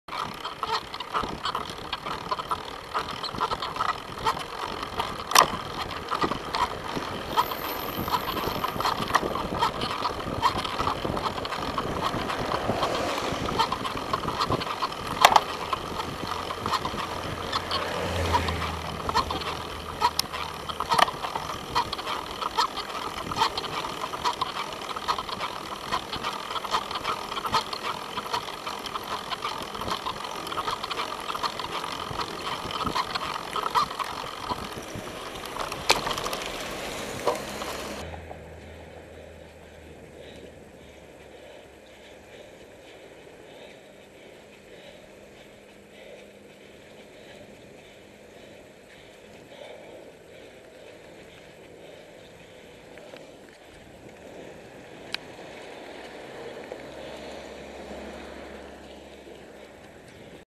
Cyclists never pull over
To be fair I rarely get the chance, drivers usually just push past!
12:30pm 16th June 2011